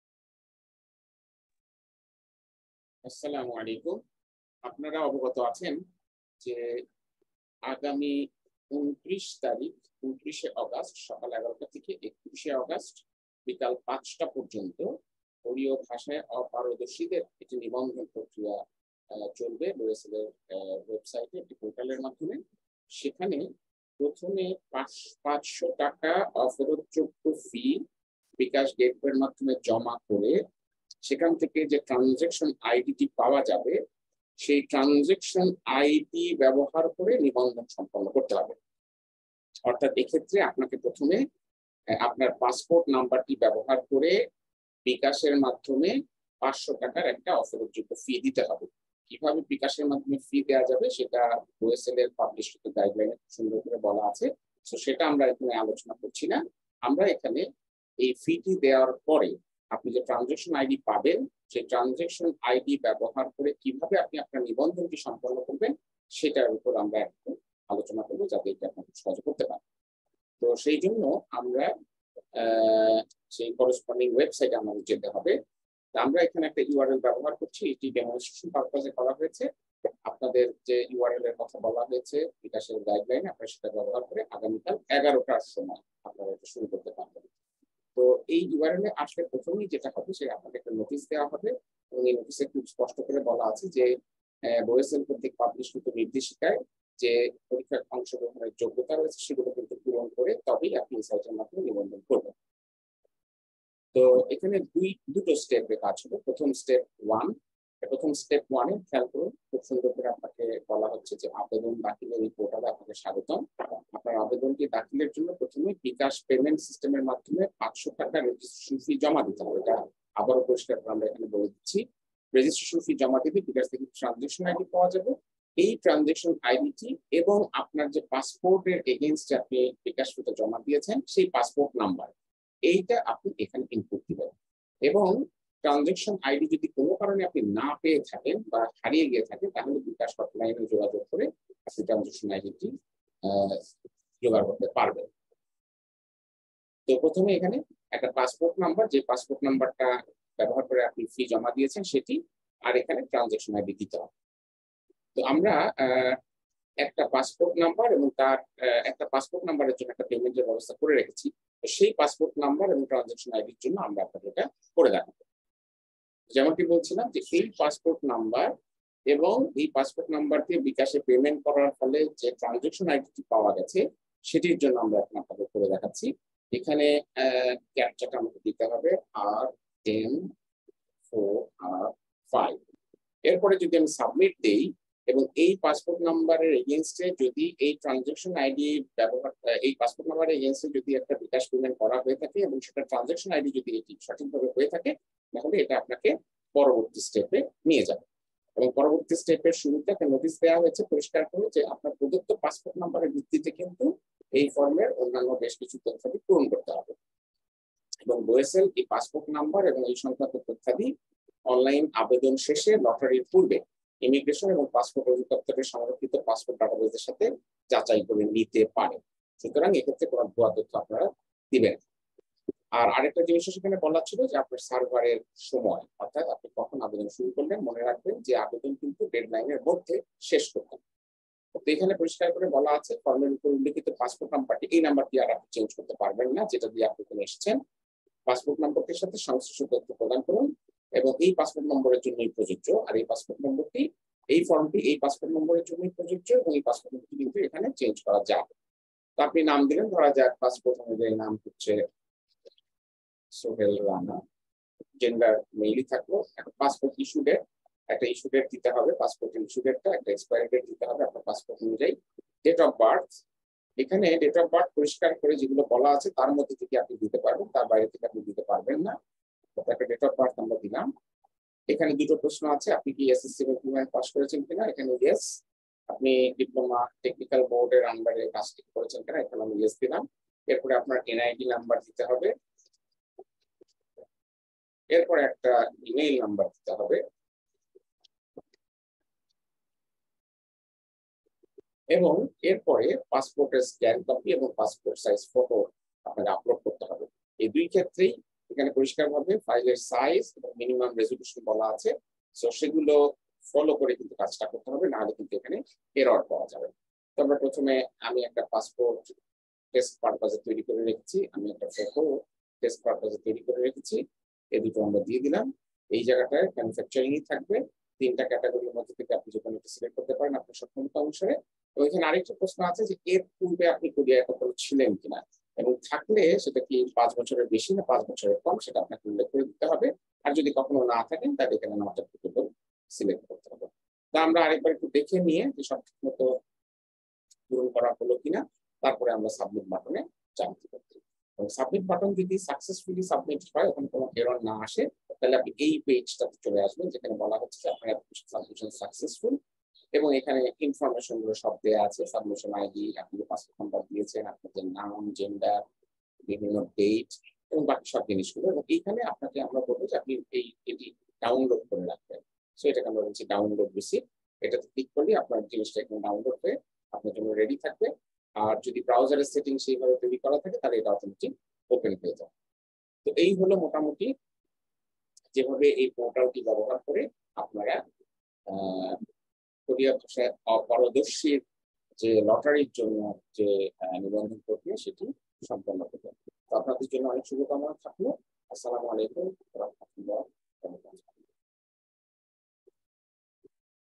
BOESL Lottery Tutorial | HRDKOREA | GOVE
কোরীয় ভাষা অপারদর্শী (লটারি) প্রার্থীদের নিবন্ধন সংক্রান্ত গাইডলাইন বা ব্যবহারবিধি সংক্রান্ত ডেমো ভিডিও .EPS.BOESL.HRD_Korea.